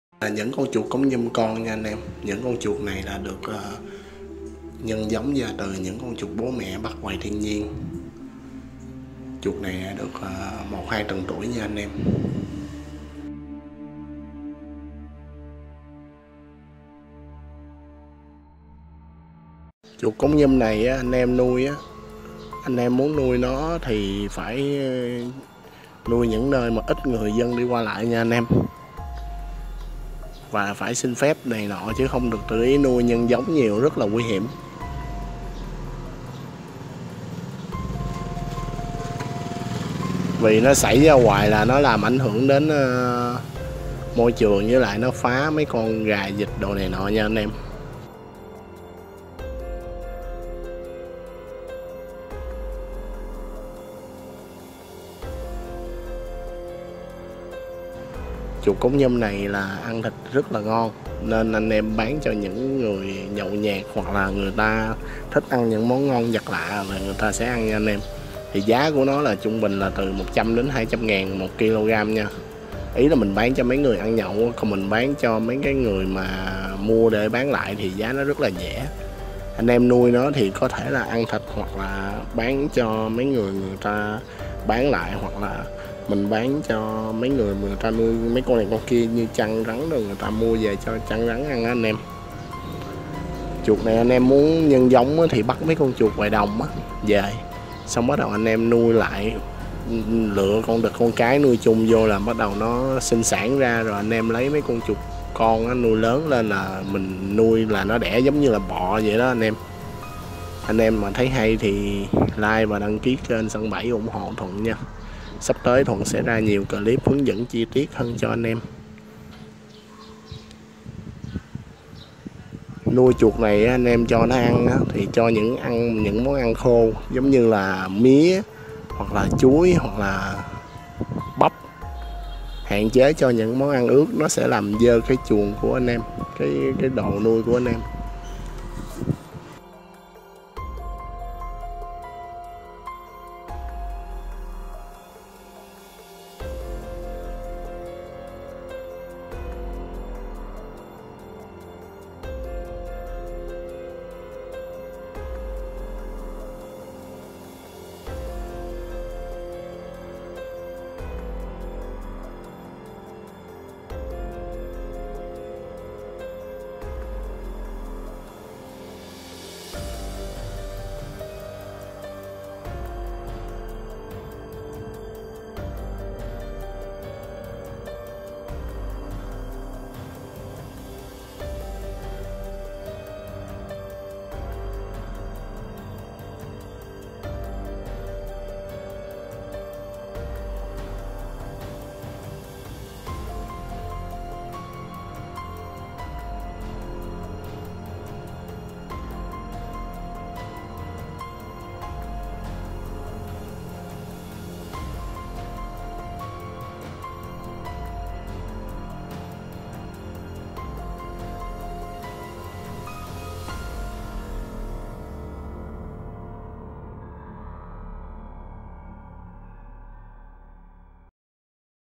Nuôi Chuột Cống Nhum Kiếm Tiền - 10 Năm Sau Kiếm Hơn 2 Tỷ Đồng
Nuôi dơi lấy phân 1 tháng kiếm bộn tiền .
Loại này ae cho nó ăn rất dễ . Nếu nuôi nhiều thì có thể lượm cơm . Đồ ăn thừa từ những quán ăn bỏ ra về lọc lại cho nó ăn . Nhưng ae muốn sạch sẽ chỗ nuôi nhốt thì cho ăn những món ăn nào khô ko ẫm ướt . Thậm chí như thuận cho a trái cây như chuối khoai lan bắp và nhiều loại khác.
Nhưng ae lưu ý loại chuột này nếu ae nuôi nhiều thì tốt nhất nên cách ly xa khỏi khu dân cư đông người và . Tránh tình trạng lỡ nó xảy ra gây ảnh hưởng đến con người . Loại chuột này
Nuôi chuột cống kiếm tiền , chuột cống nhum nuôi kiếm tiền ,